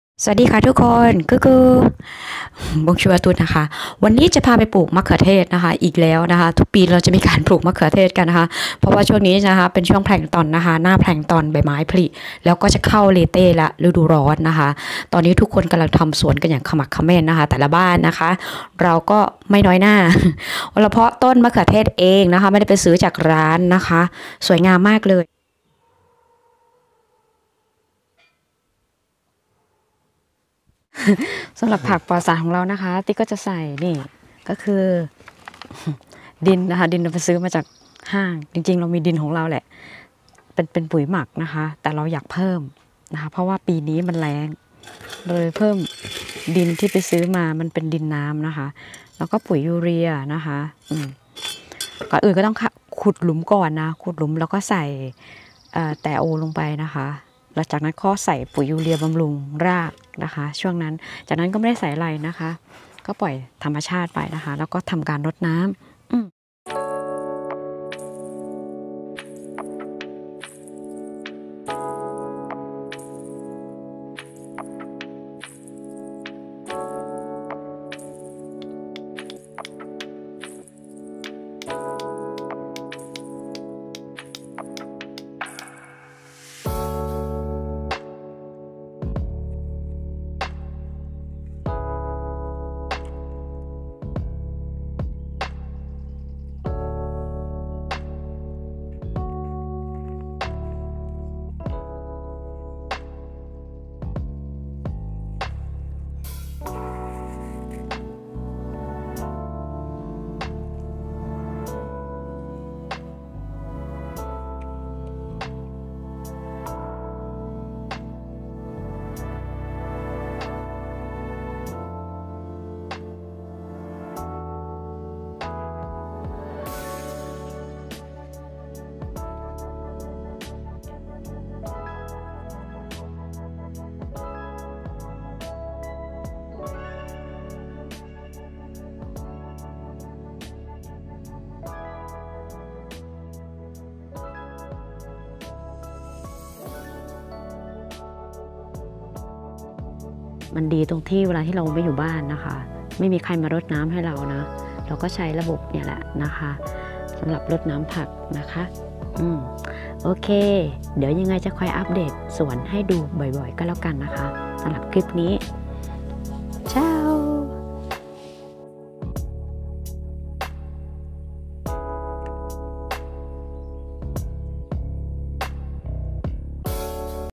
ได๋เวลาปลูกผักอีกแล้วจร้าาา
ฤดูกาลปลูกมะเขือเทศที่ฝรั่งเศส